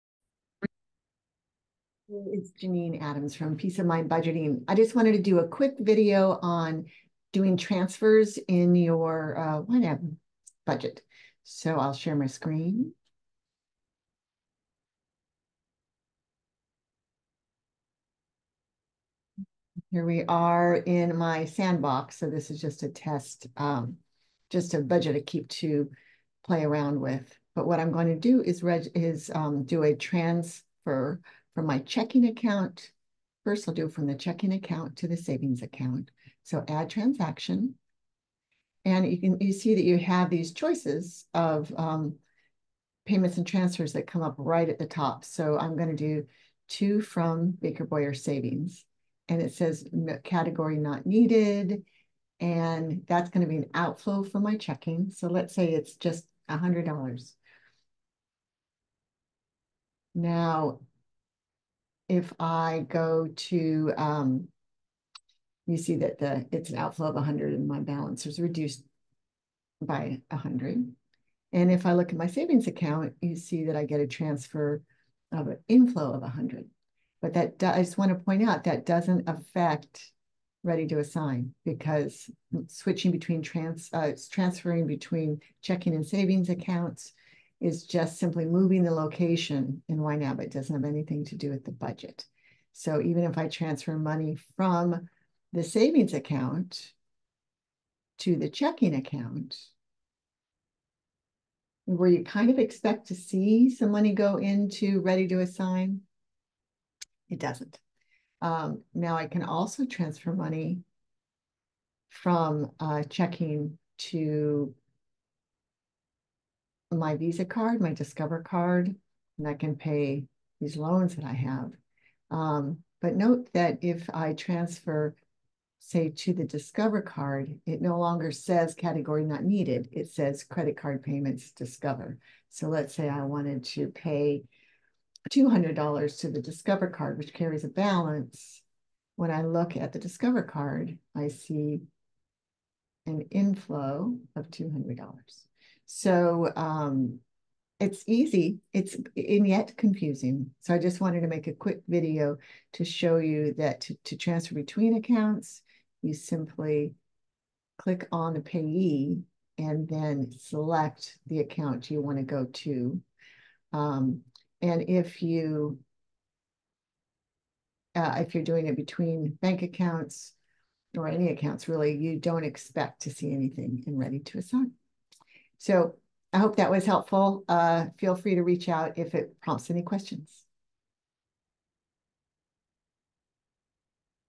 Entering transfers in ynab
Transferring money between accounts in YNAB can look confusing but it's really quite simple if you remember to change the Payee to an on-budget account. @janineadams of Peace of Mind Budgeting shows you how.